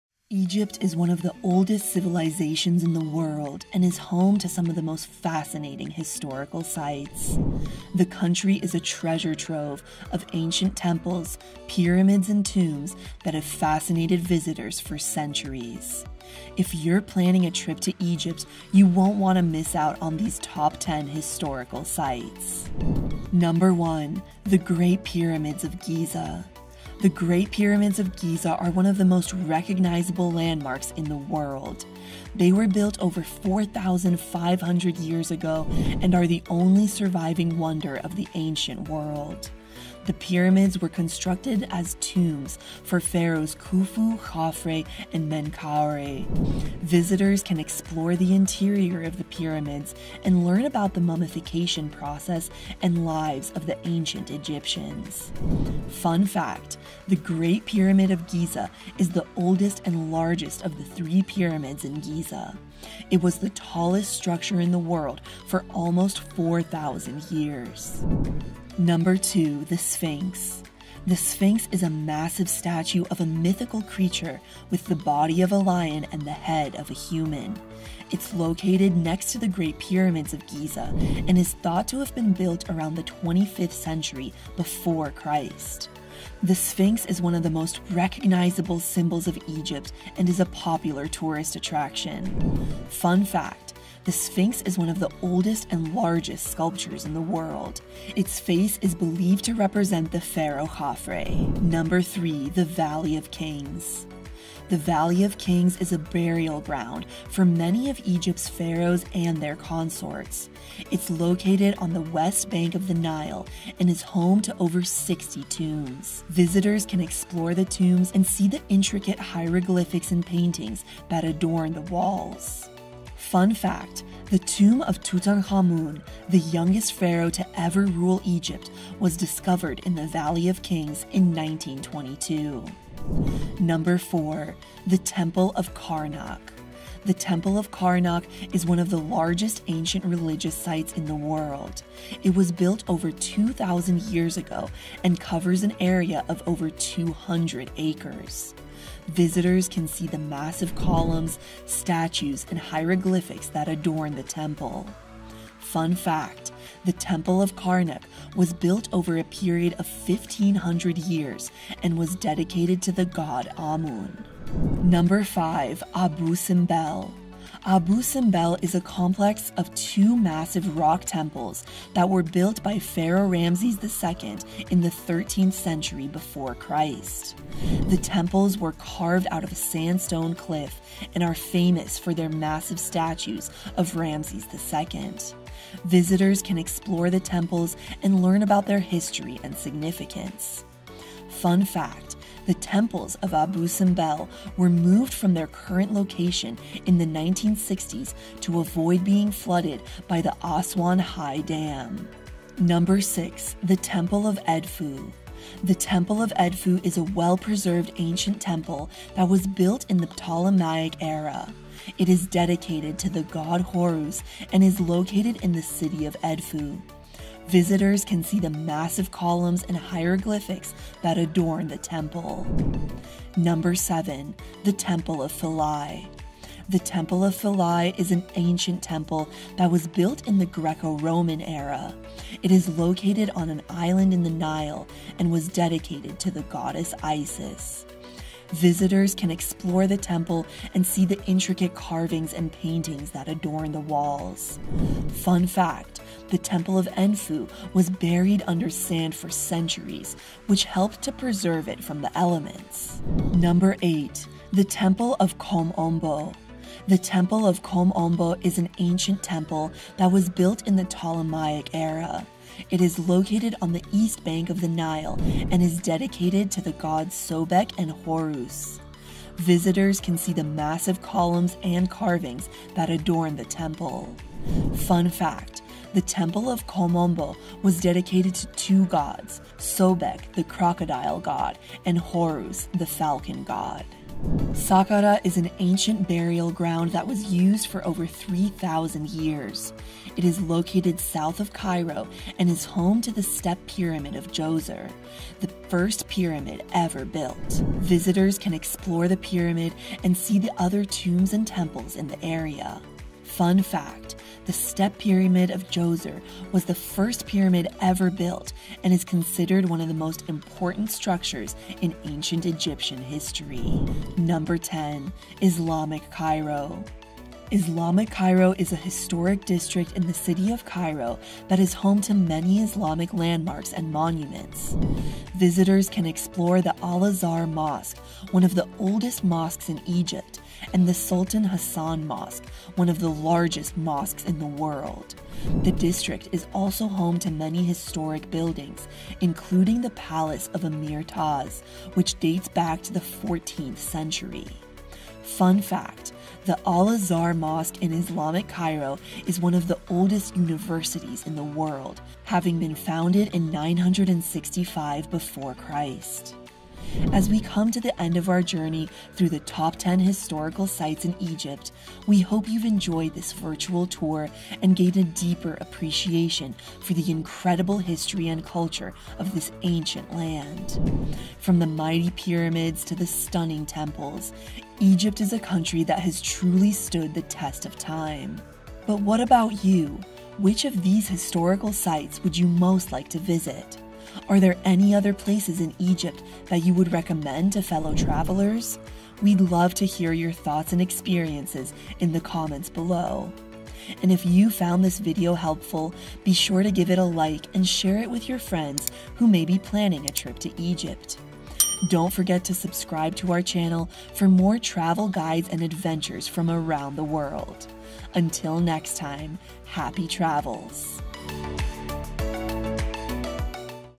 From Pyramids to Temples: Discover the Secrets of Ancient Egypt
Unlock the ancient mysteries of Egypt in our captivating journey from the magnificent Pyramids of Giza to the hidden treasures of the Valley of the Kings. Join us as we venture into the majestic Great Pyramids, standing tall for over 4,500 years, revealing the secrets of pharaohs' tombs and the enigmatic mummification process. Get up close and personal with the iconic Sphinx, guarding its riddles since the 25th century BC. Prepare to be awe-struck as we explore the colossal Temple of Karnak, a testament to the devotion of ancient Egyptians. And that's just the beginning! Discover more astonishing temples like Abu Simbel, Edfu, and Philae, each with its own tale to tell. Immerse yourself in the rich history and grandeur of Islamic Cairo, where ancient mosques and historic landmarks await your exploration. Embark on this unforgettable adventure, and uncover the timeless wonders of Ancient Egypt!